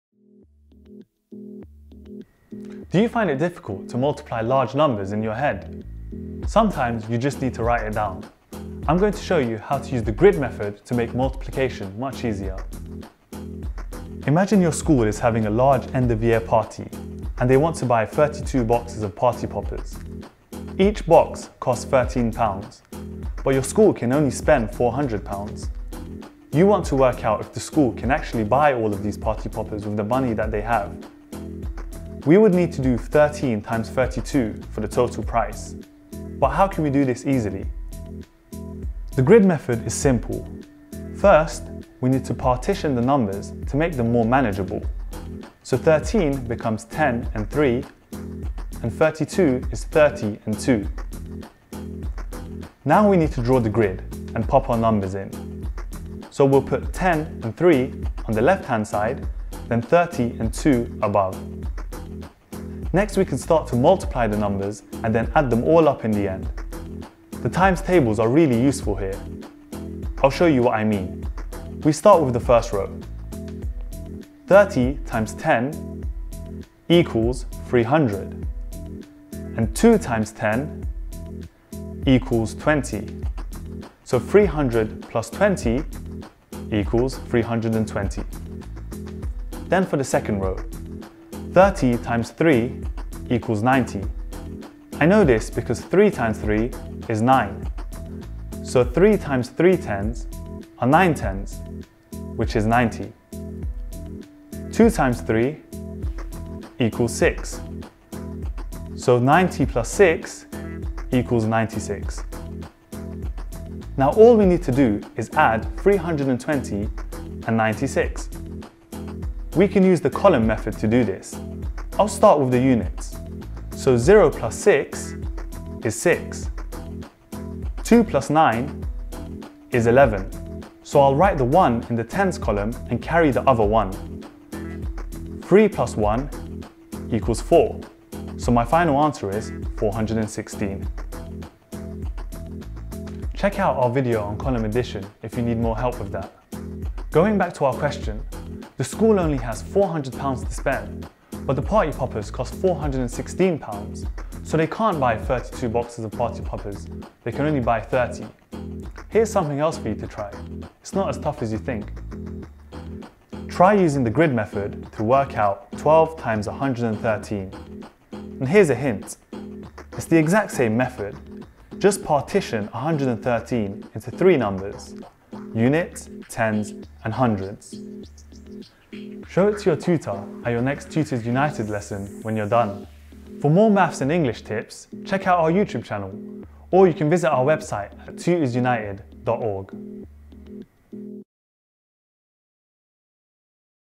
Grid Multiplication
Do you find it difficult when you need to multiply large numbers? It's much easier to write it down and our tutor Ali knows a method that will really help you out.